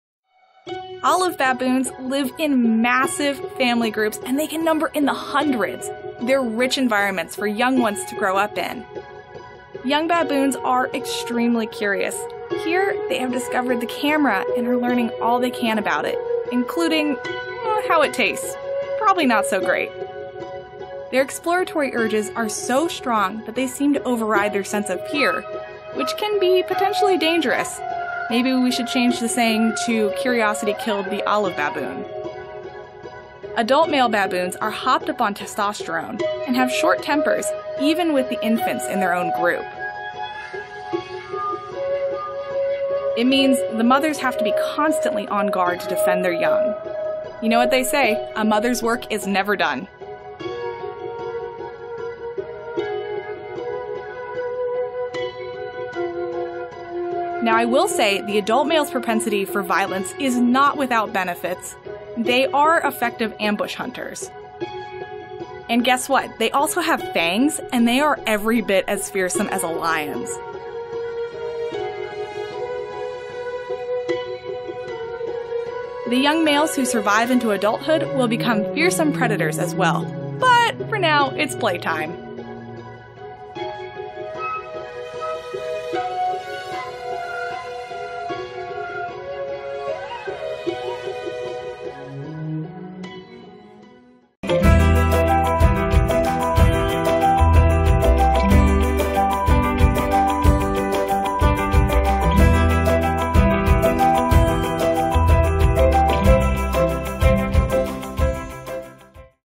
Olive the Other Baboons Are Curious | Nat Geo Wild
Olive baboons live in massive family groups that can number in the hundreds.